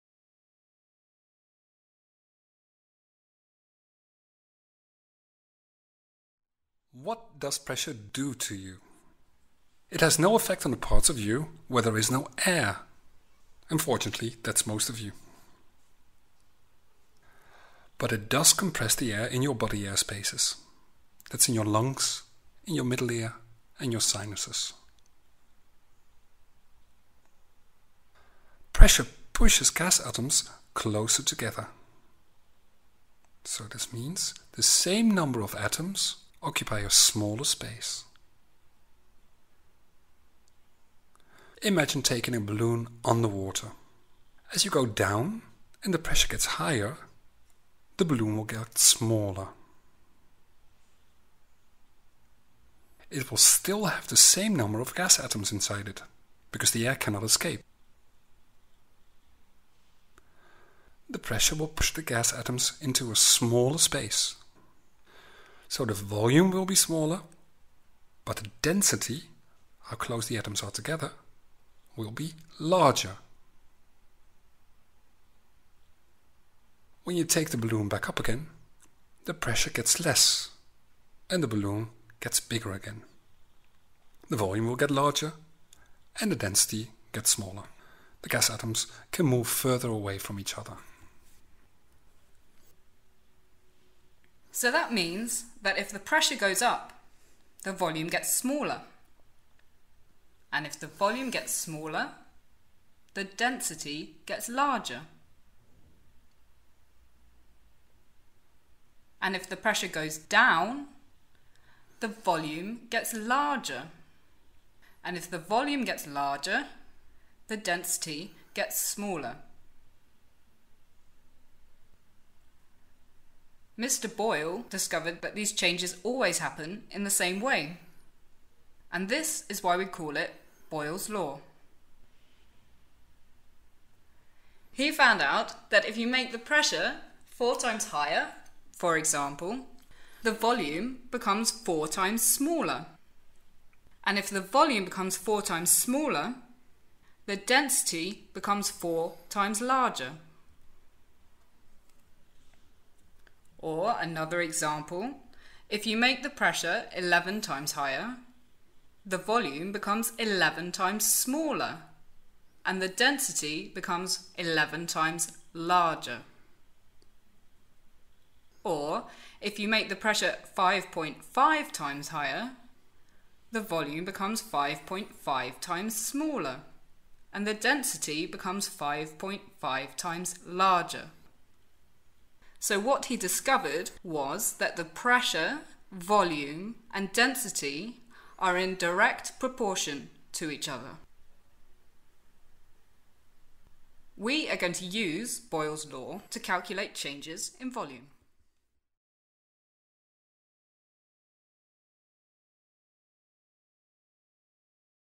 Boyle's Law
A video lesson. Boyle's law: the realtionship between pressure, volume and density.  For example, if the pressure  goes up, volume goes the same  amount down.